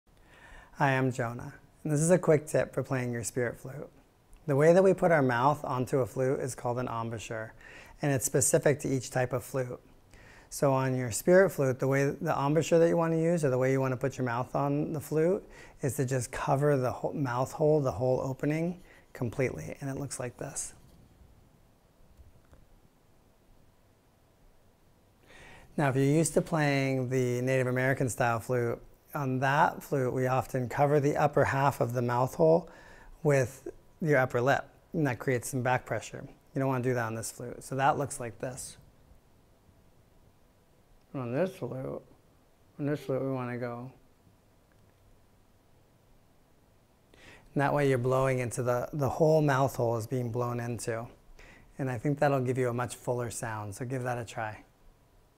How to put your mouth on the Spirit Flute - Using an embouchure - Spirit Flute Playing Tips
Playing Tip: There is a specific way to put your mouth on a flute, it's called an embouchure.
Using the correct embouchure on a Spirit Flute will make a big difference in the quality of the flute's sound. And the embouchure for a Spirit Flute is different than for a Native American Style Flute!